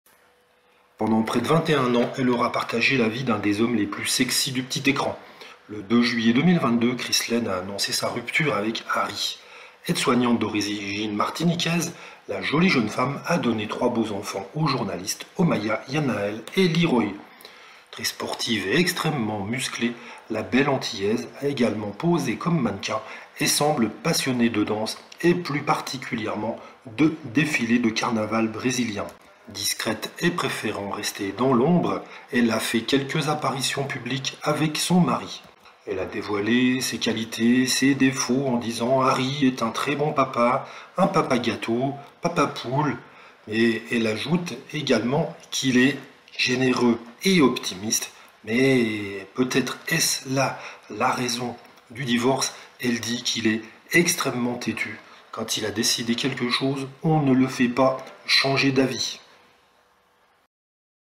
Harry Roselmack : la raison du divorce dévoilée
Le beau journaliste de 49 ans est de retour sur le marché des célibataires depuis que son épouse Chrislaine a annoncé leur séparation. Belle Antillaise très athlétique, la jeune femme semble aussi belle à l'intérieur qu'à l'extérieur